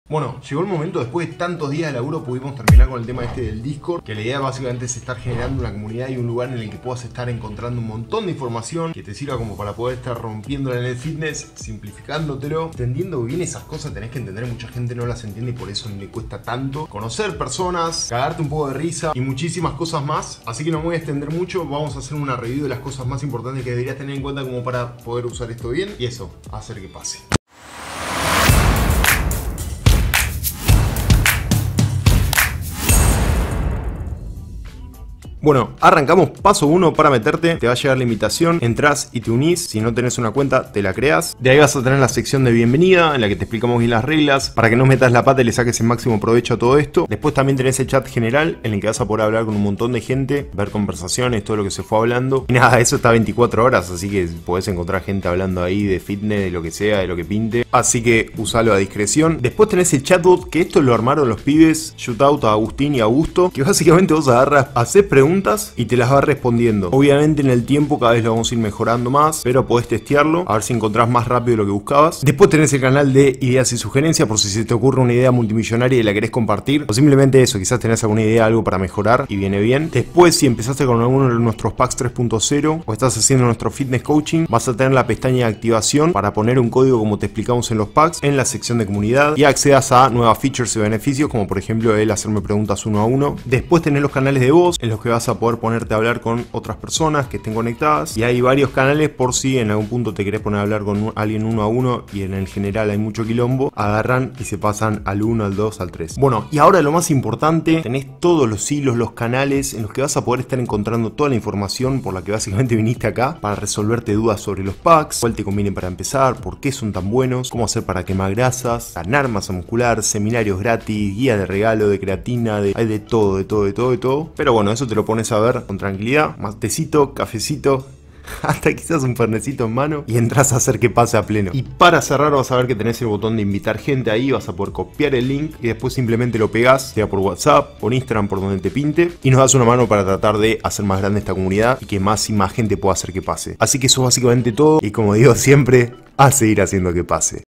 Uopak Discord: mini introducción
Bueno! Llegó el día y queríamos hacérsela un poco más fácil a las personas que quizás no están muy familiarizadas con esta plataforma!

Surgió del problema de no poder llegar a contestar en tiempo y forma tantos mensajes y creemos que puede llegar a ser una gran solución!

Índice:
00:00 Palabras iniciales

00:35 Bienvenida

00:46 Chat General

00:59 Chat-Bot!

01:13 Ideas & Sugerencias

01:20 Activación de Usuario 3.0

01:33 Canales de Voz

01:46 Hilos de información fitness

02:11 Invitar gente